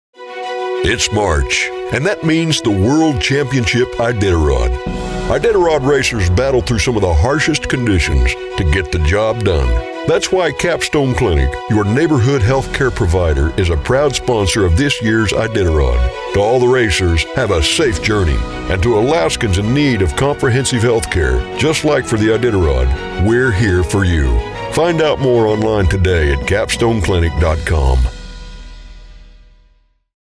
Capstone is a Proud Sponsor of the Iditarod
Capstone Clinic is Alaska's leading family healthcare and urgent care provider with clinics and testing centers across the state. Find out more at CapstoneClinic.com